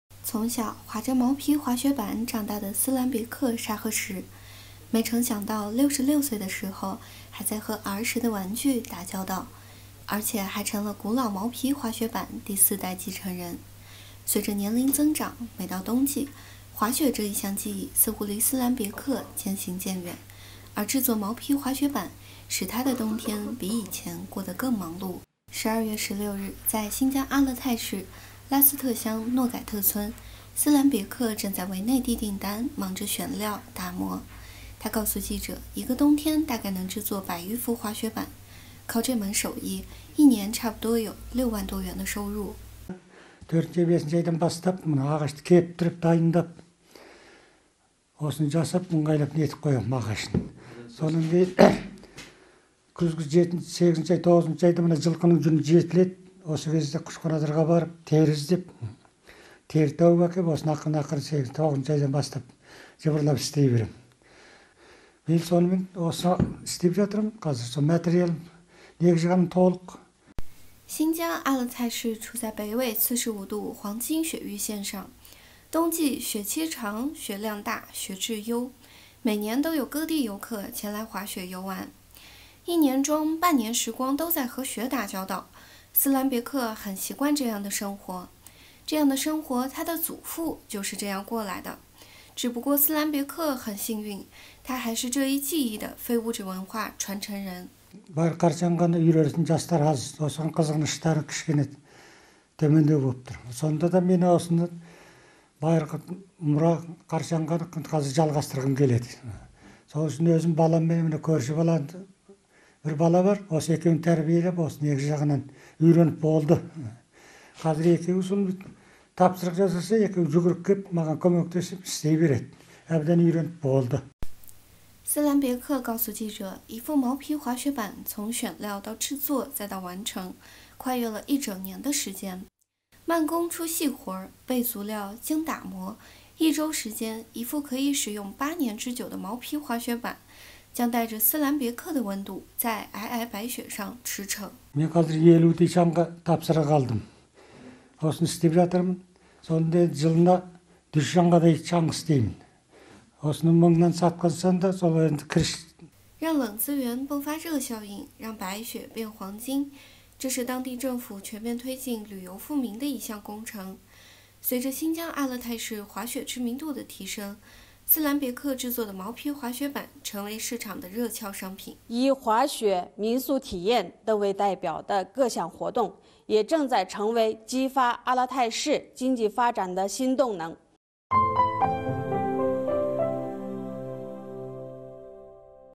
新疆阿勒泰：维族老人打造手工滑雪板
从小滑着毛皮滑雪板长大的斯兰别克·沙合什，没成想到66岁的时候还在和儿时的玩具打交道，而且还成了古老毛皮滑雪板第四代继承人。随着年龄增长，每到冬季，滑雪这一项技艺似乎离斯兰别克渐行渐远，而制作毛皮滑雪板使他的冬天比以前过得更忙碌。